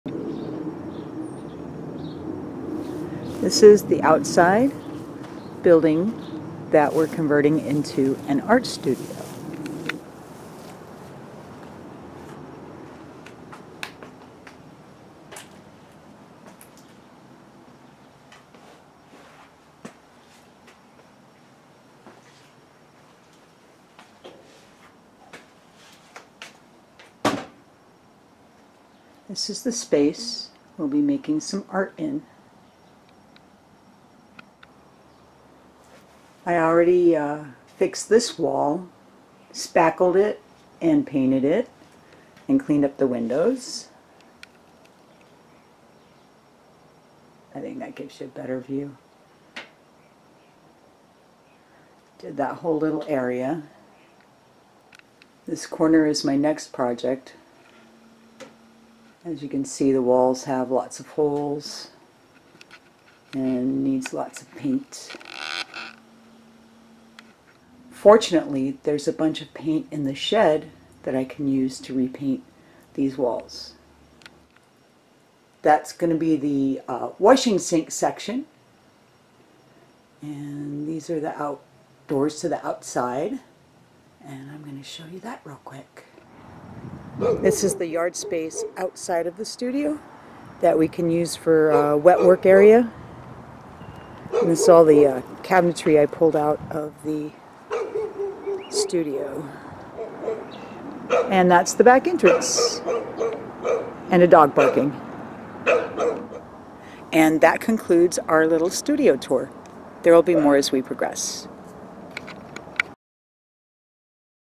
Art Studio
Re-modeling a workshop into an art studio.